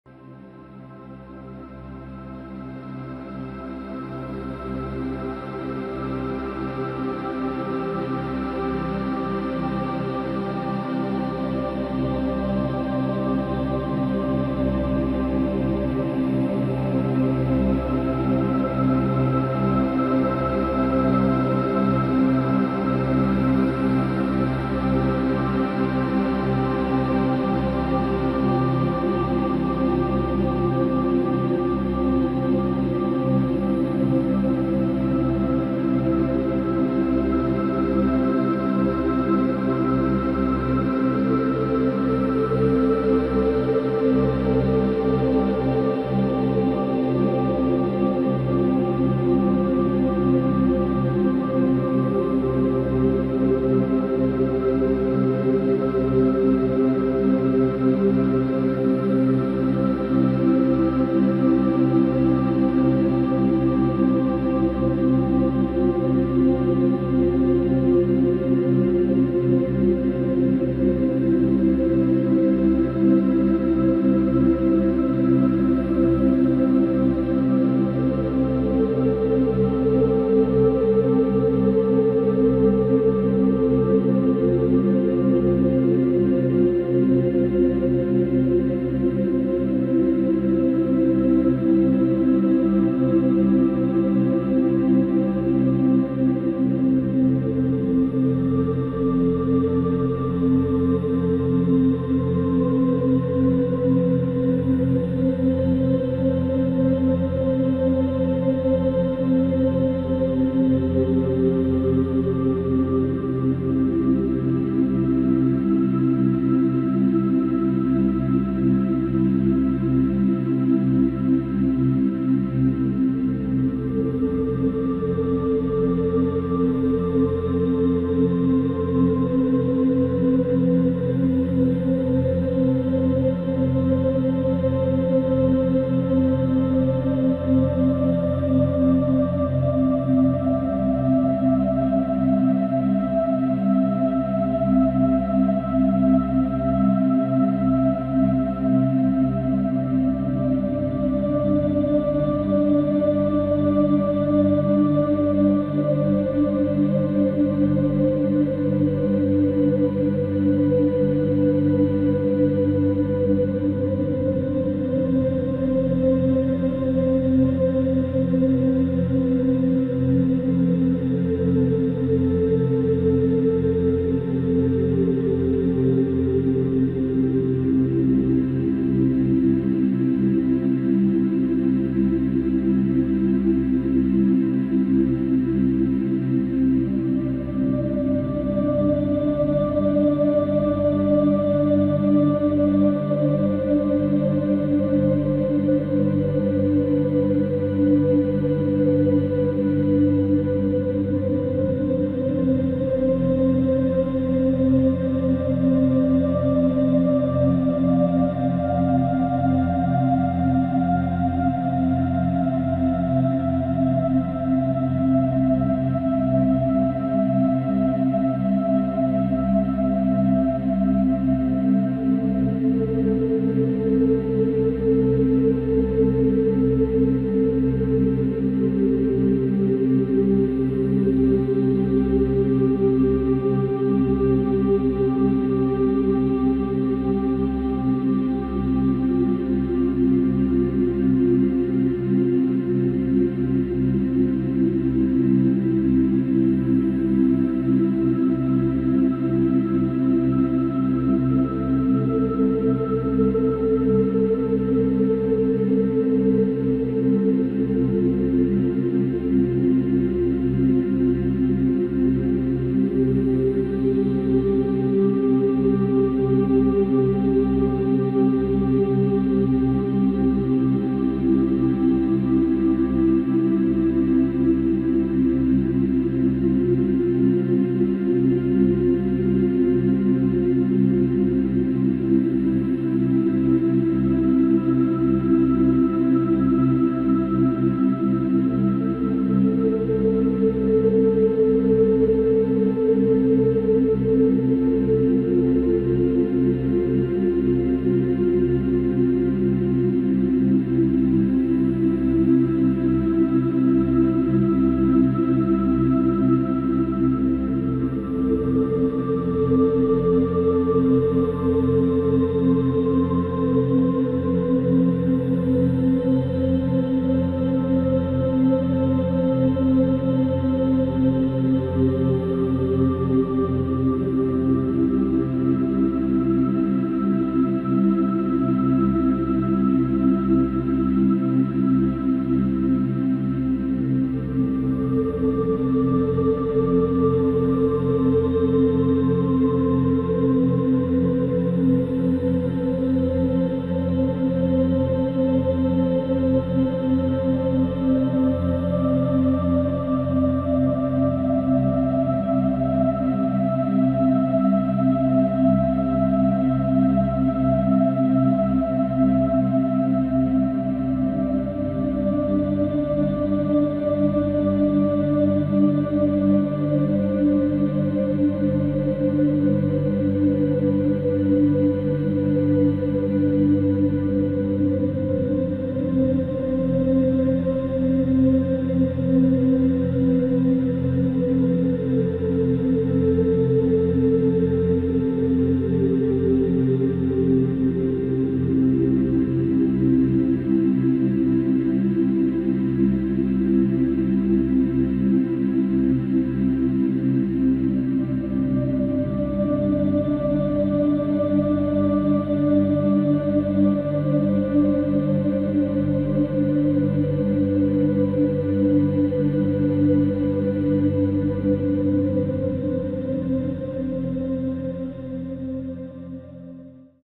Digital Speed Painting - Fantasy Landscape - Rift
This one felt like a bit of a grind at times, struggling with lack of direction but I just about managed to push through it.